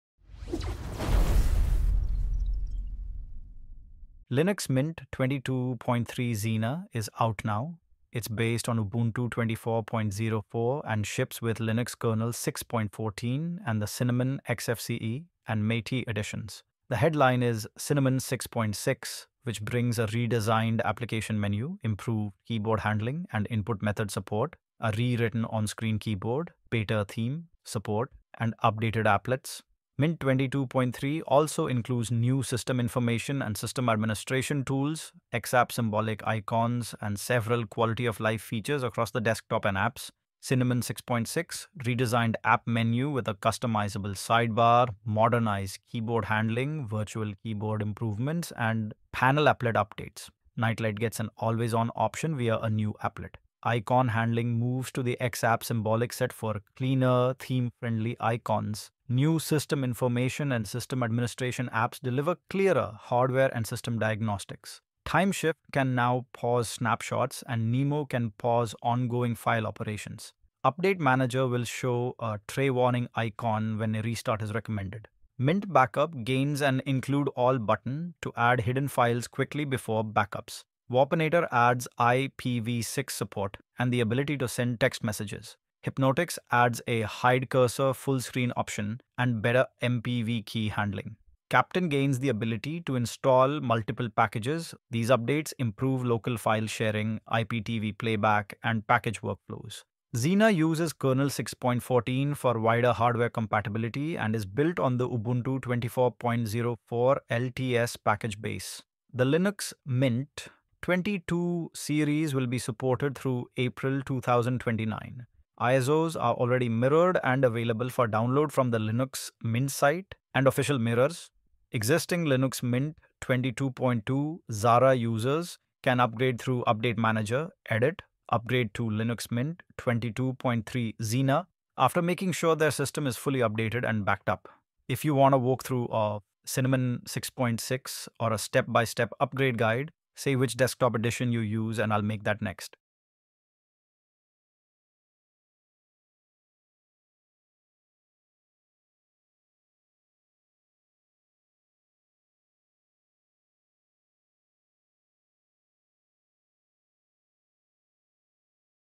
Linux Mint 22.3 “Zena” Released | Cinnamon 6.6, Kernel 6.14 & New Features Explained
Linux Mint 22.3 “Zena” is officially available for download, bringing major updates based on Ubuntu 24.04 LTS. In this video, we cover everything new in Linux Mint 22.3, including the Cinnamon 6.6 desktop, Linux kernel 6.14 for improved hardware support, new System Information and Administration tools, XApp symbolic icons, Night Light improvements, Timeshift snapshot controls, Nemo file manager updates, and enhanced apps like Warpinator and Hypnotix.

You’ll also learn how existing Linux Mint 22.2 users can upgrade safely to Linux Mint 22.3 “Zena”, what editions are available, and how long this release will be supported. If you’re a Linux Mint user or planning to switch from Windows, this update is worth checking out.

🔹 Based on Ubuntu 24.04 LTS
🔹 Cinnamon, Xfce, and MATE editions
🔹 Supported until 2029